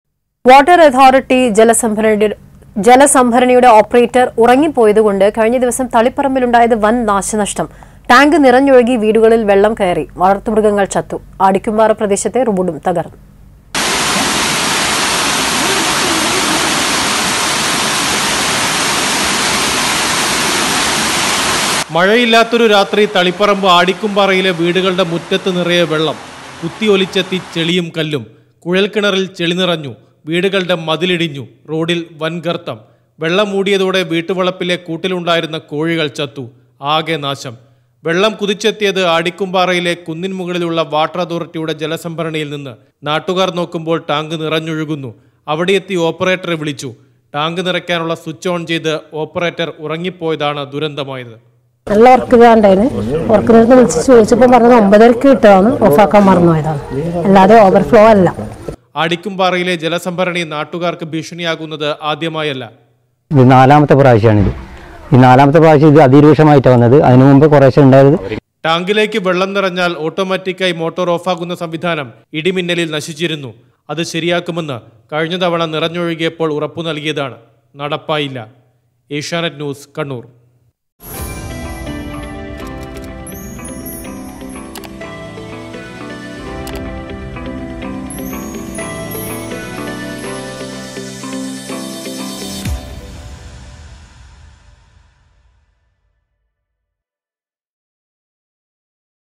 മോട്ടർ ഓൺ ചെയ്ത് ഒന്ന് ഉറങ്ങിപ്പോയി, ശേഷം കാഴ്ചയിൽ ഇങ്ങനെ | Kannur
മോട്ടർ ഓൺ ചെയ്ത് ഓപ്പറേറ്റർ ഒന്ന് ഉറങ്ങിപ്പോയി; വെള്ളം കുത്തിയൊഴുകി വളർത്തുമൃ​ഗങ്ങൾ ചത്തു, റോഡ് പൊളിഞ്ഞു; ദുരിതമയം